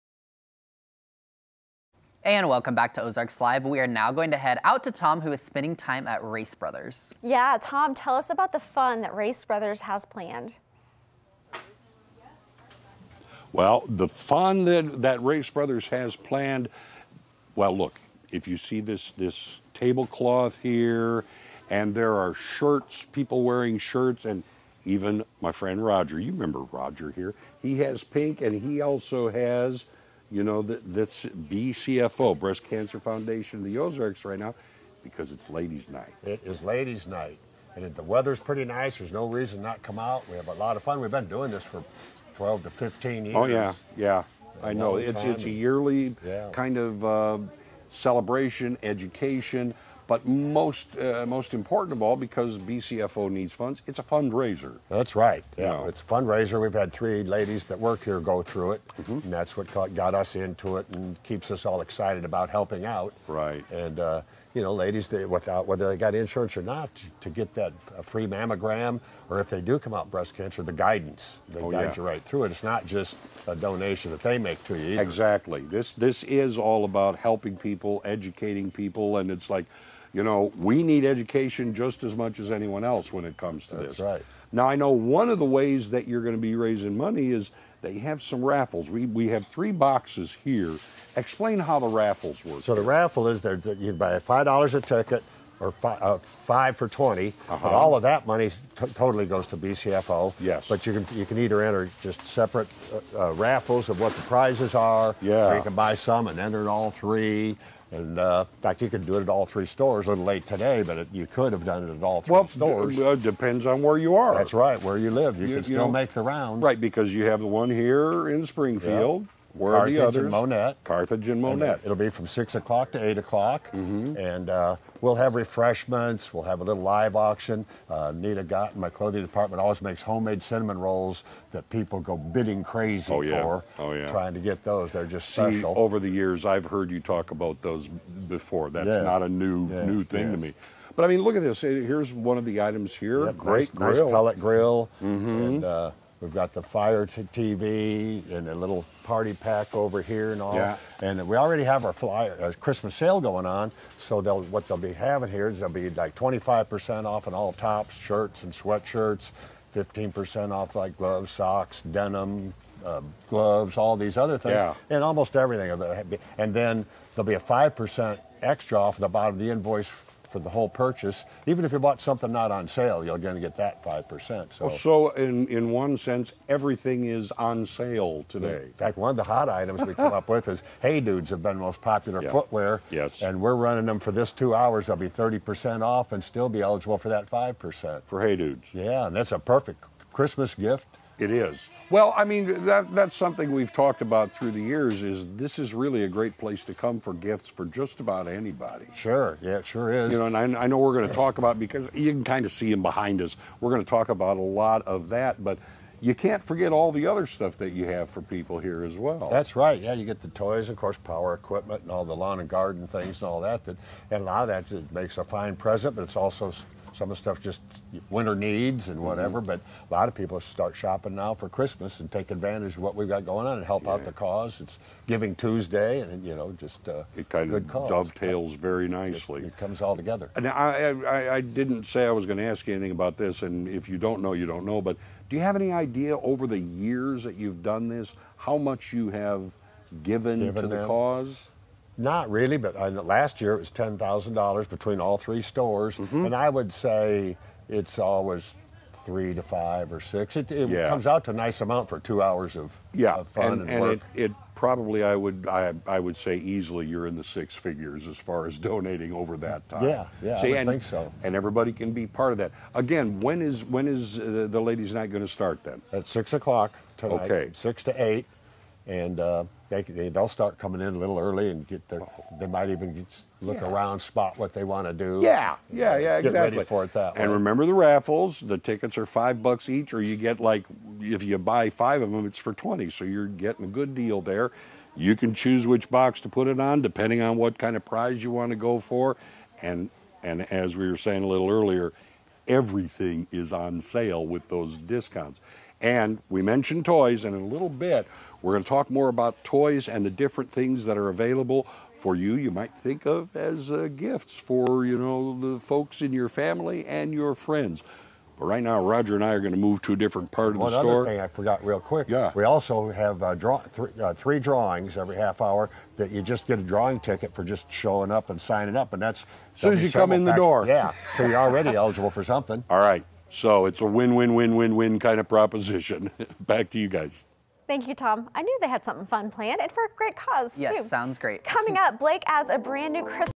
Ladies Night at Race Brothes Farm & Home Supply
Tom Trtan visits Race Brothers Farm & Home Supply to get all the necessary details on their
ladies night event.